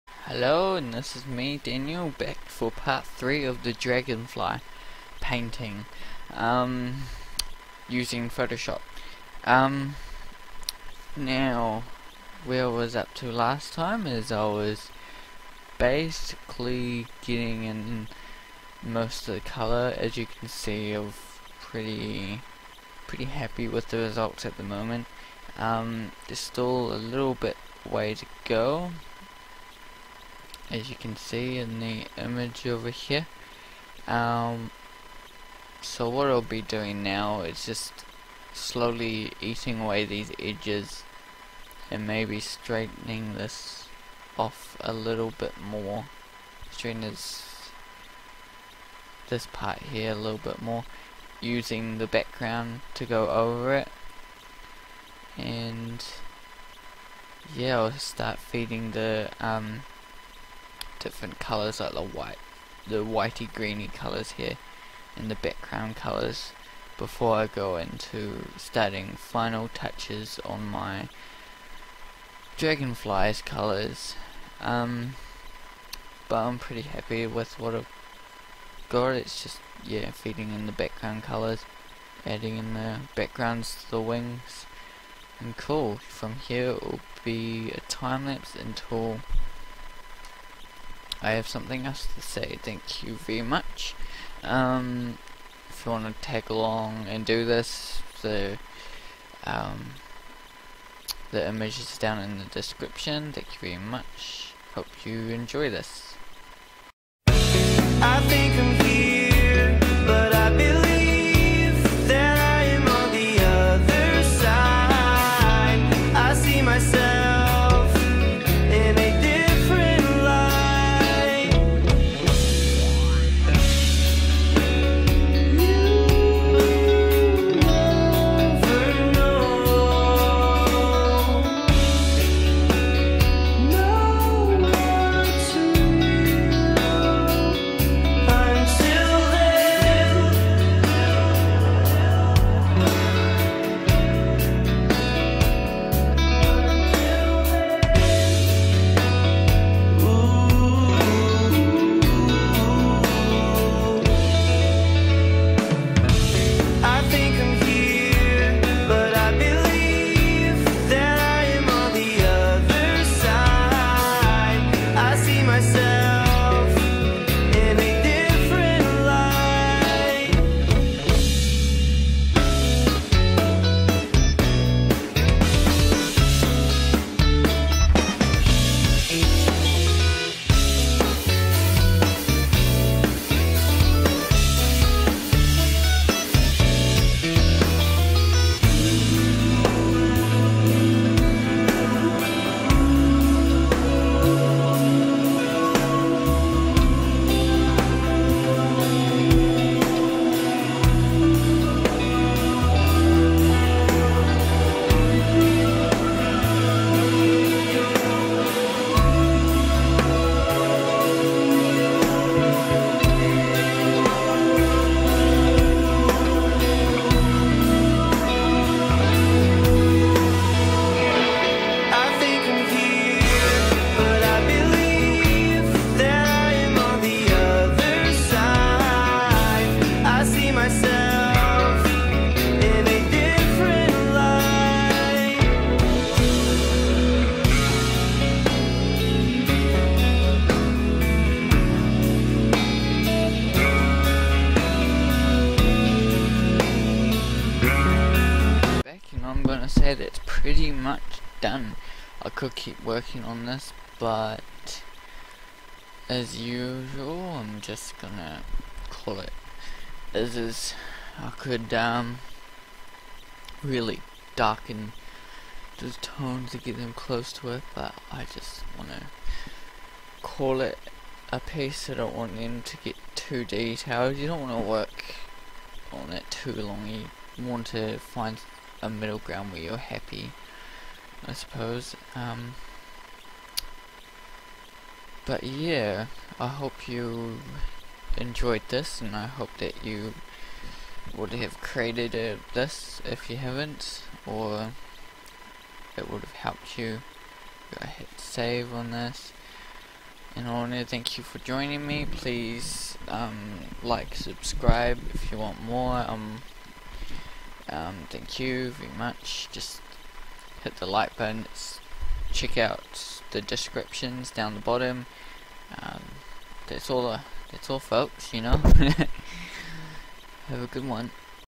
Digital Drawing with kingcheet: Dragonfly part 3
This is the third part of me digitally painting this dragon fly.
If you would like to follow along from part 1 and 2 you can get the image